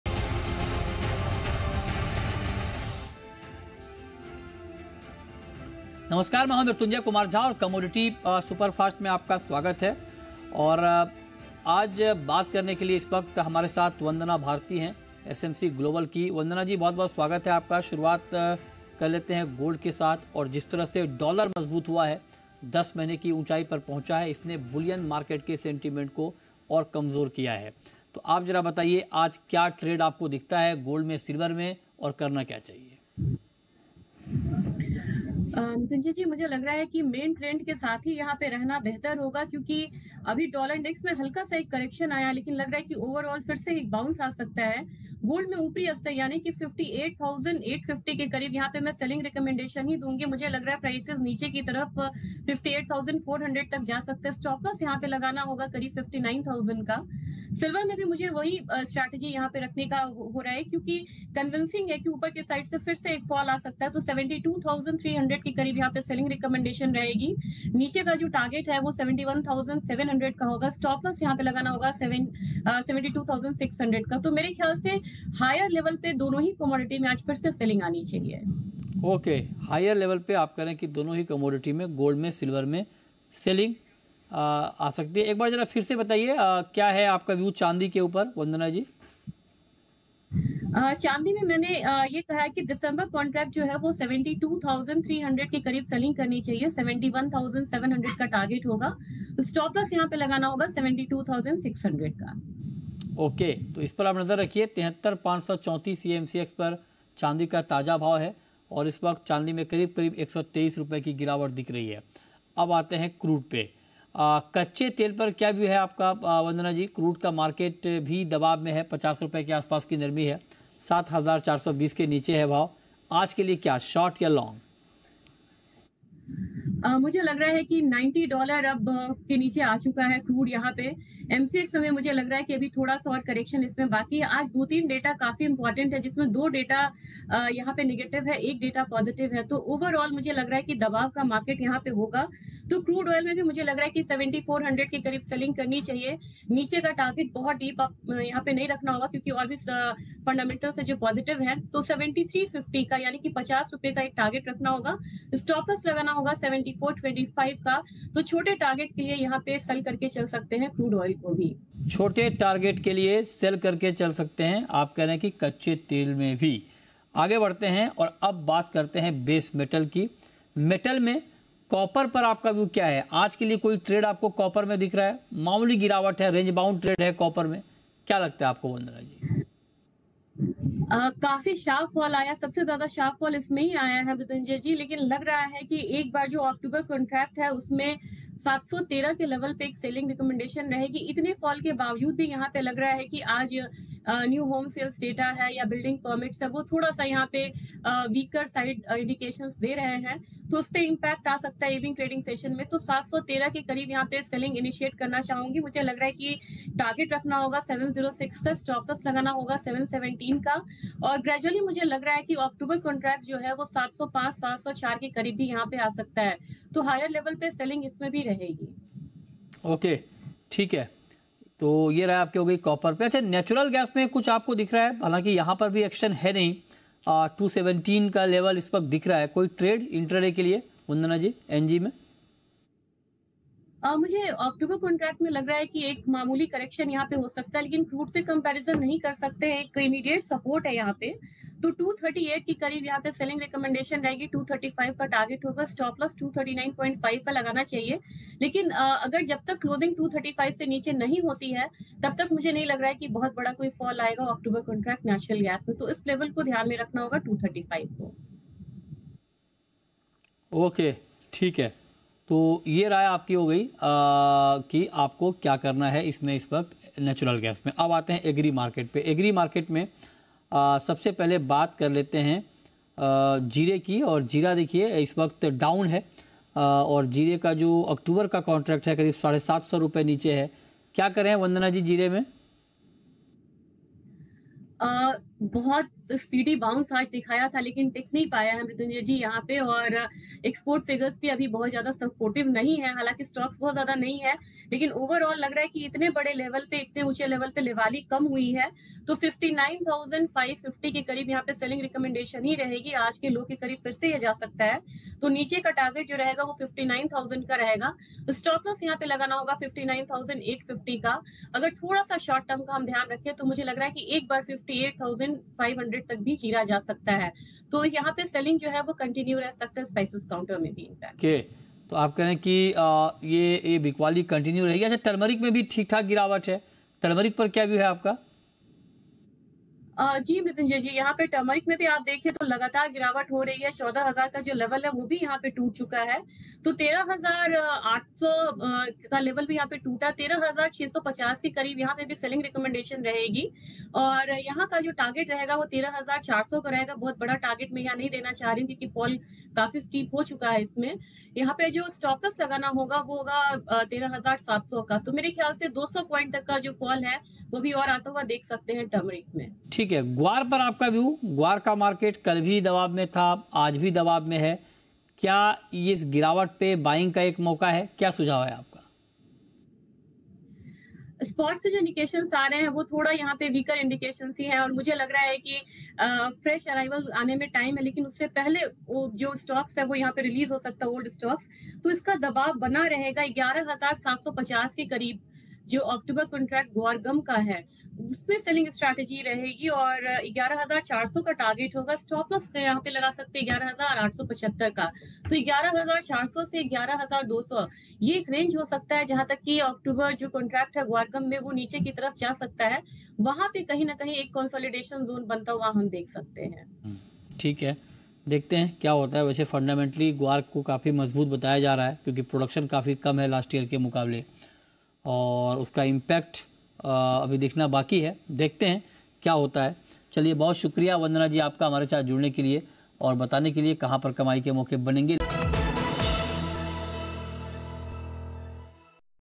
Commodity Superfast: MCX पर सोना ₹58700 और चांदी ₹72100 के नीचे हुए बंद| Gold-Silver Price
Gold-Silver Price: मंगलवार के शेयर बाजार में सोने-चांदी में काफी दबाव देखने को मिला, सोना ₹58700 के नीचे पहुंचकर बंद हुआ तो वहीं चांदी ₹72100 के नीचे बंद होते हुए नजर आया

Gold-Silver Price: There was a lot of pressure on gold and silver in the stock market on Tuesday, gold closed below ₹ 58700, while silver was seen closing below ₹ 72100.